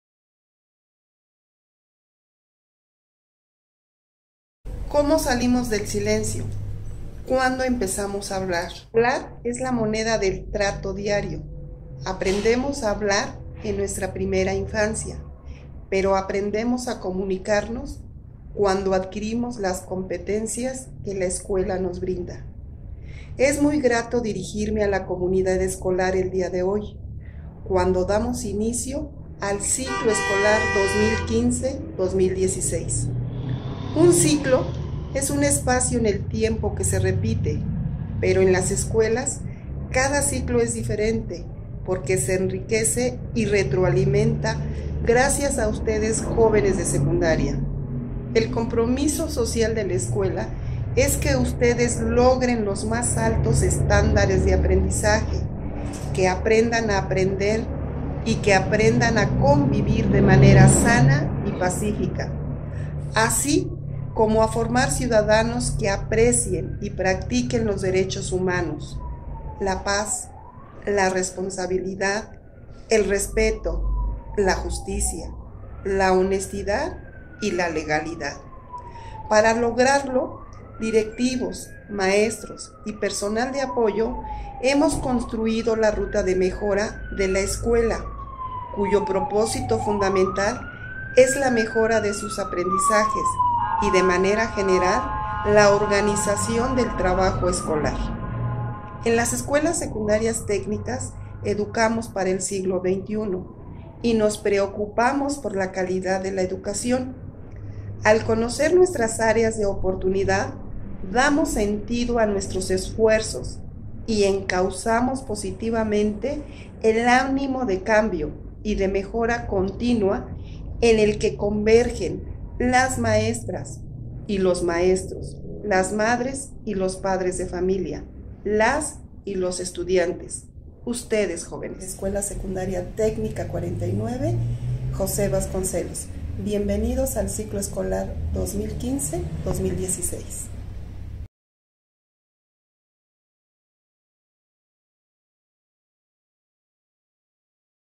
20150826-CE1516/Bienvenida de Directores/EST 49
Bienvenida de Directores. EST 49. Directora: Maria de los Ángeles Pedraza Miranda. Este y otros programas los puedes ver con fines educativos para secundarias técnicas en el Distrito Federal, México.